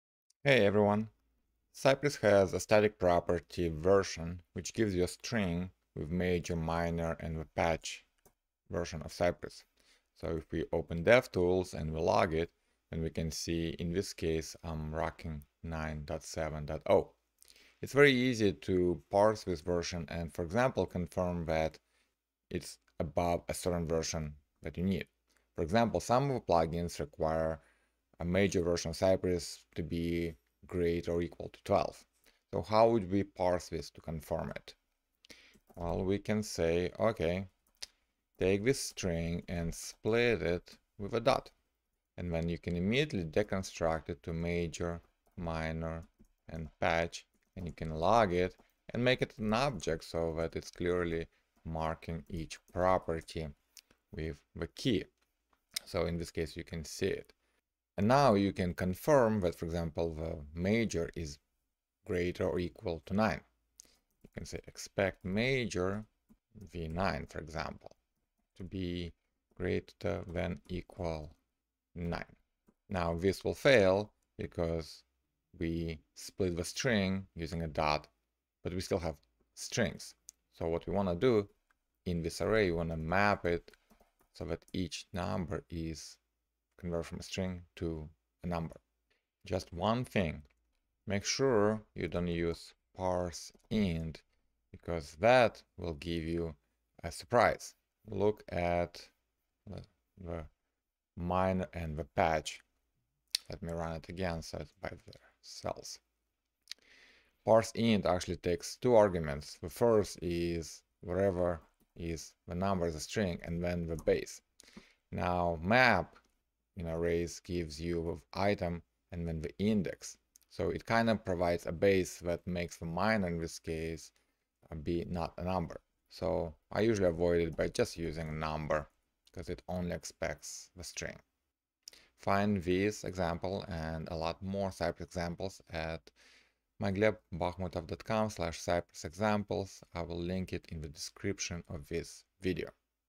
Parse Cypress Version String And Confirm The Major Version
Sometimes you need to confirm the Cypress version used to run the specs. For example, my plugin cypress-map only works with Cypress v12 or later. This short video explains how to parse the Cypress.version string

console.log(Cypress.version)
const [major, minor, patch] = Cypress.version
  .split('.')
  .map(Number)
console.log({ major, minor, patch })
expect(major, 'v9 requires').to.be.gte(9)